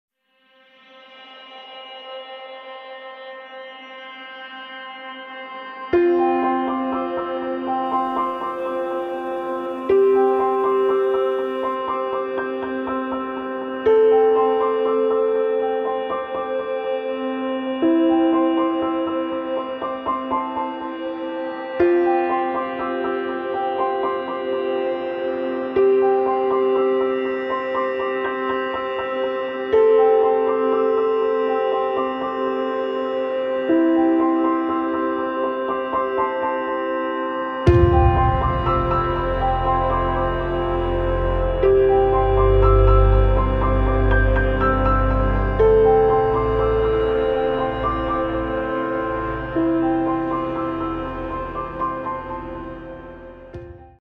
Claire and Iain 29.12.17 - Teaser Trailer
A teaser trailer of the wedding of Claire and Iain Gavin at Corinthian in Glasgow on 29th December 2017